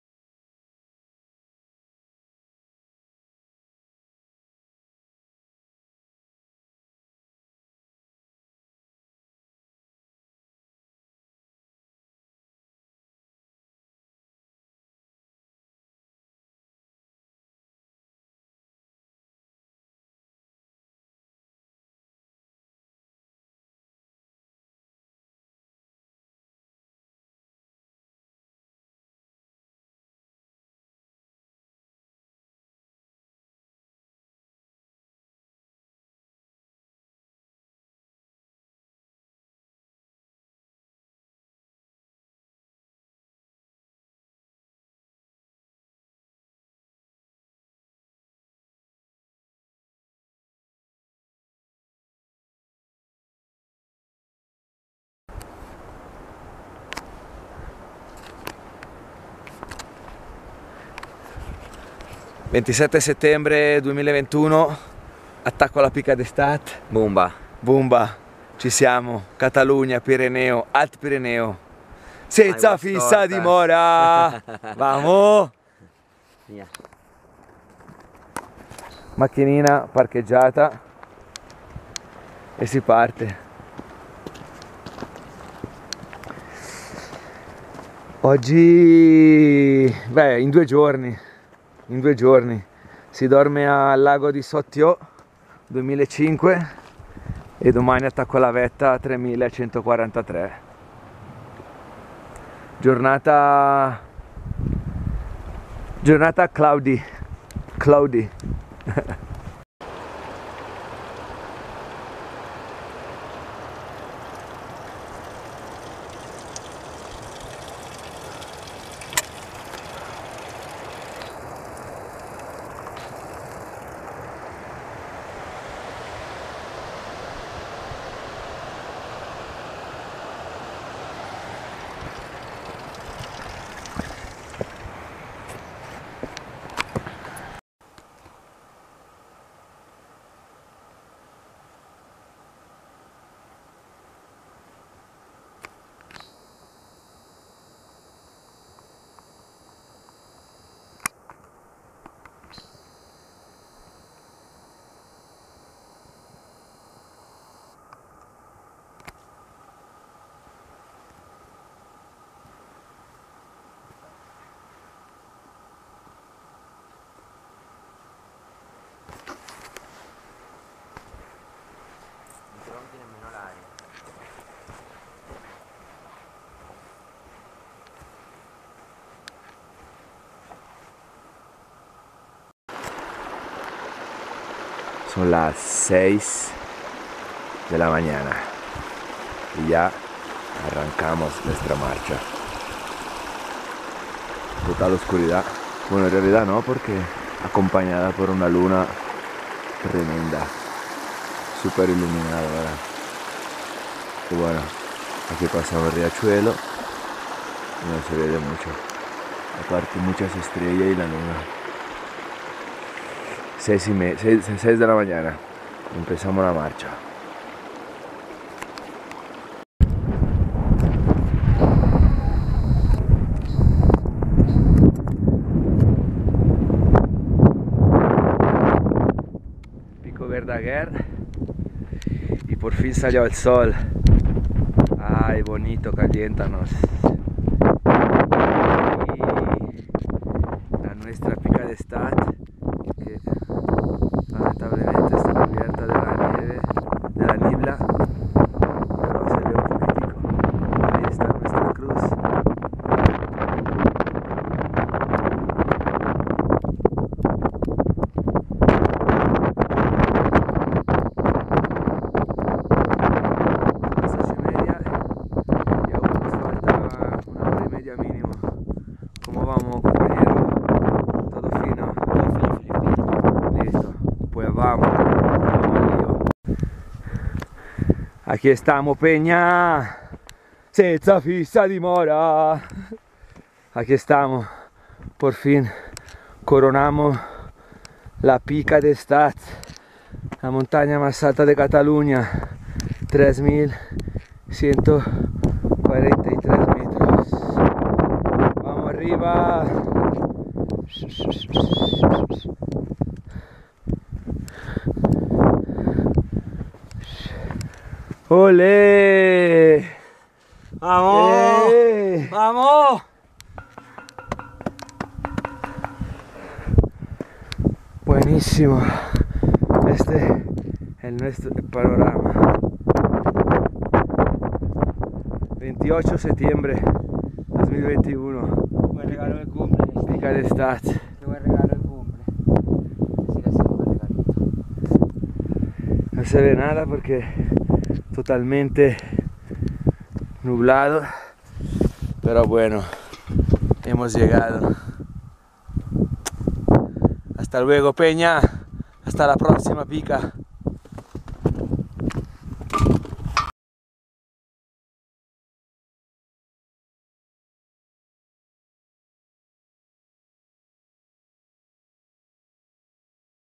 💥 PICA D'ESTATS 3143mt. & PICO VERDAGUER 3129mt. 💥
🧗‍♀️  28/29 Septiembre 2021 🧗‍♀️

Pica d'Estats & Pico Verdaguer:

-Aparcament la Molinassa 1804mt.
-Refugi Vallferrera 1940mt.
-Estany de Sotllo 2350mt.
-Pica d'Estats 3143mt.
-Pico Verdaguer 3129mt.